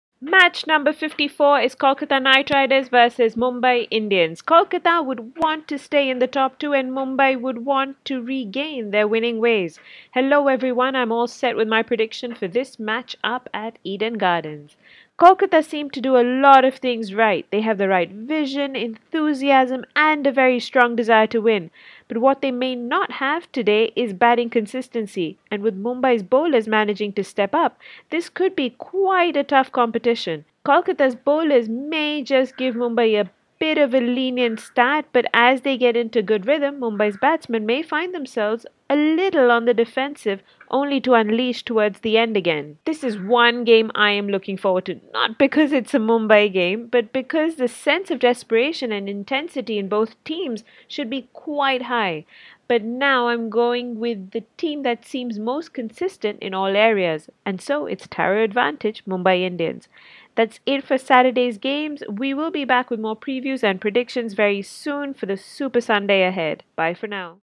IPL 10 - Kolkata Knight Riders v Mumbai Indians - Match 54 Prediction 13th May 2017
Check out Shruti's IPL prediction for the 54th game - Kolkata Knight Riders v Mumbai Indians that's to be played on 13th May 2017.

Photo credit: Suneer Chowdhary